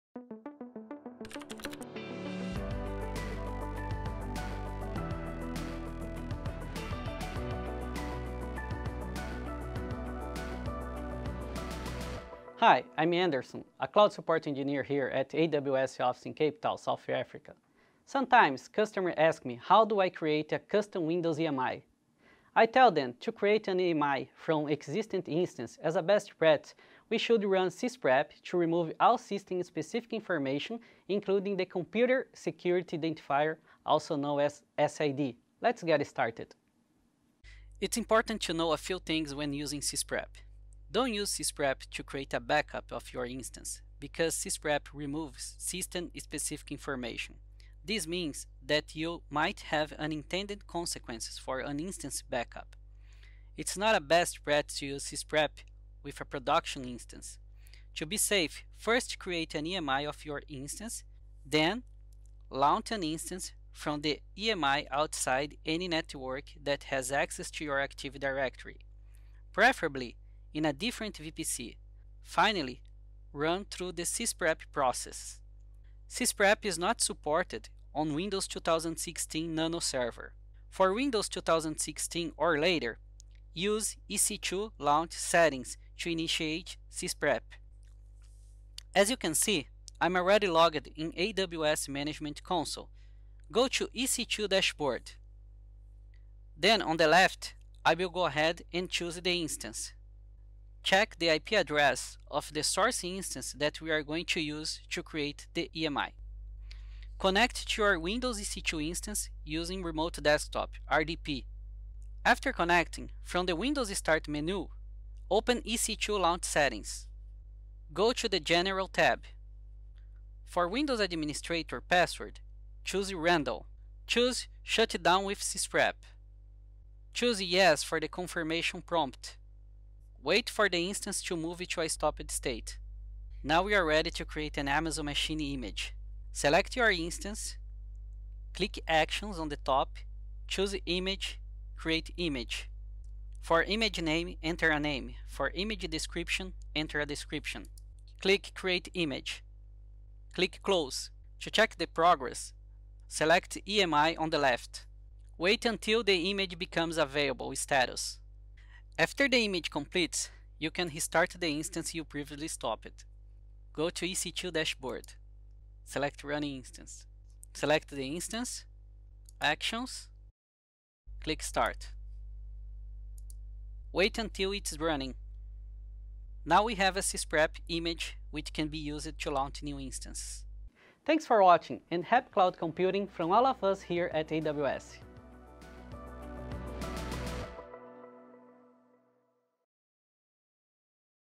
Use Sysprep to create and install custom reusable Windows AMIs for Windows Server 2016 and later.
For more details see the Knowledge Center article with this video
Anderson shows you how to use Sysprep to create and install custom reusable Windows AMIs for Windows Server 2016 and later.